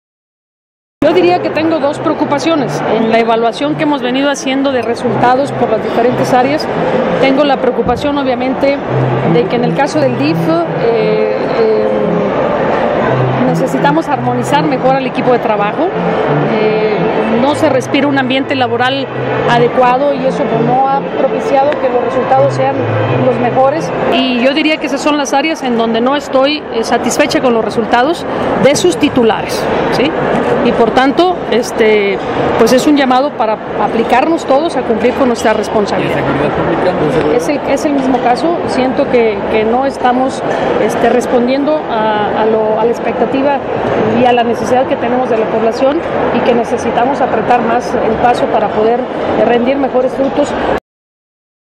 ANUNCIA LORENA MARTÍNEZ PROBABLES CAMBIOS EN DIRECCIONES DE SEGURIDAD PÚBLICA Y DIF
La alcaldesa capitalina Lorena Martínez Rodríguez manifestó que luego de las evaluaciones efectuadas a tres meses de su administración, probablemente se lleven a cabo cambios en Seguridad Pública y DIF, cambios que pueden ser, según los resultados, desde las cabezas, ya que a decir de la alcaldesa "son las áreas en las que no estoy satisfecha de los resultados de sus titulares y por tanto es un llamado para aplicarnos todos a cumplir con nuestra responsabilidad".